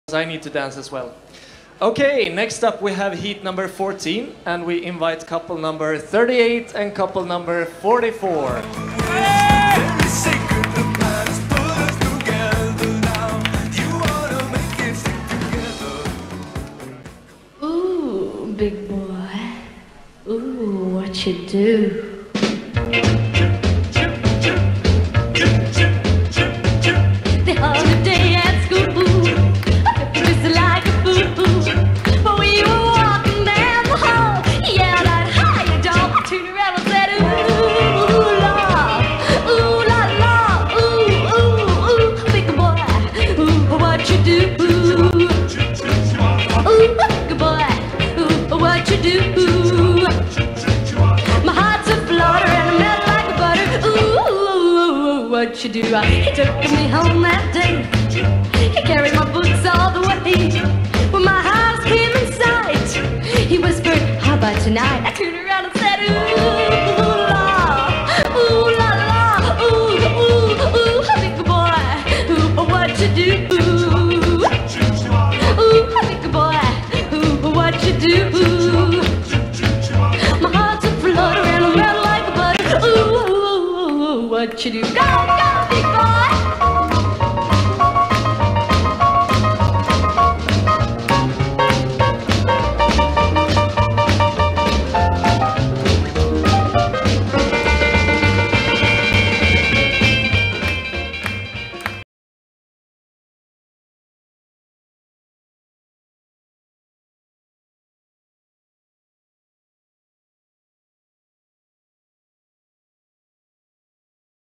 World Cup Zürich: WRRC Boogie Woogie Main Class - First Round Fast - Alberto & Giulia
Crazy World Cup Zurigo - Primo Round - WRRC - Boogie Woogie Fast